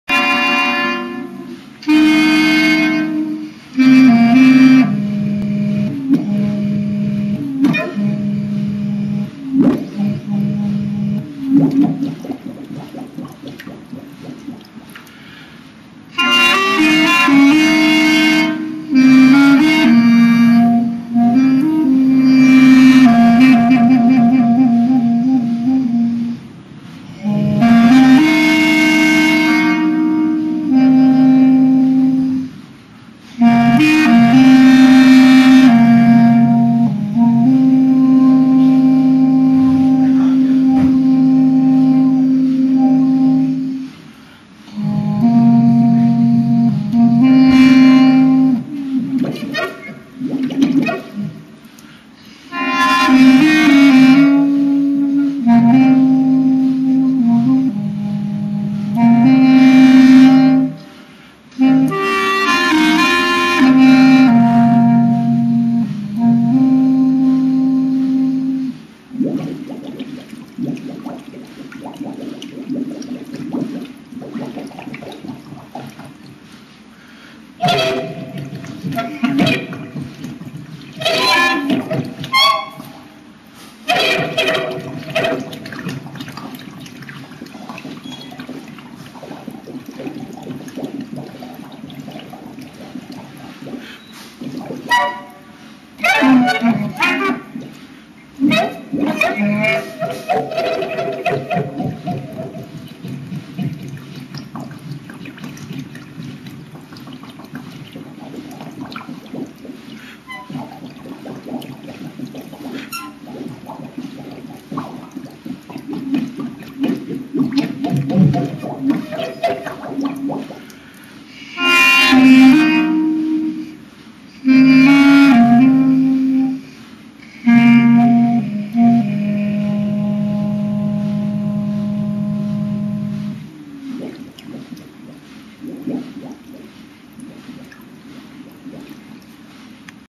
BLUES AND UNIVERSAL CLARINET - CLEMATRIX " J'IRAI JOUER CHEZ VOUS " MUSIQUES DES MONDES
CLEMATRIX dans " j'irai jouer chez vous " s'imprègne du lieu où il va se produire, les sons servent de points de passage de notre quotidien ...  une bassine d'eau  avec la clarinette transformée en un univers différent pour nous faire  entendre un Water Blues  Lofi